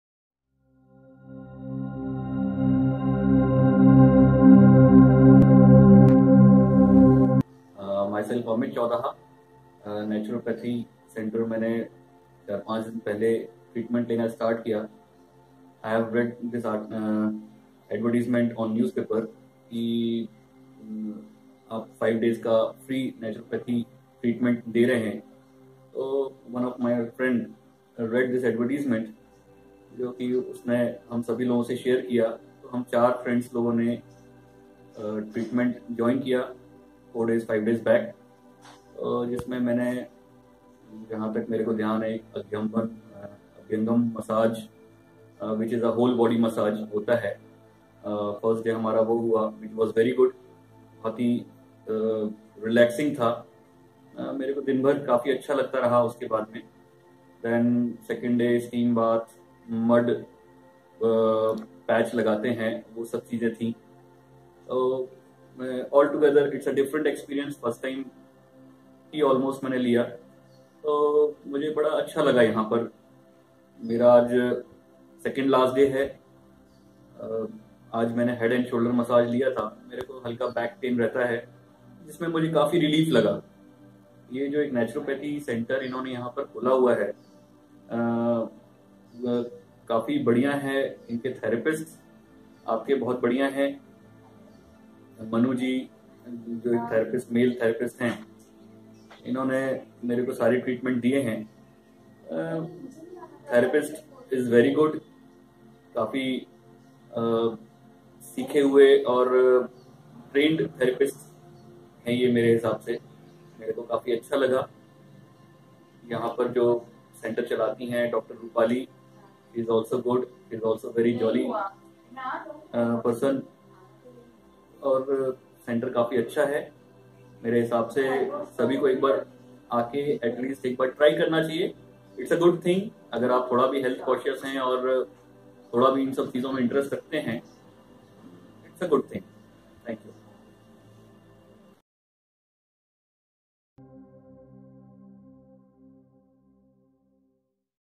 Our clients Mr Amit Choudha Feedback about Naturopathy Camp in Katni
Our clients Feedback about Naturopathy Camp
Call for appointment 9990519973, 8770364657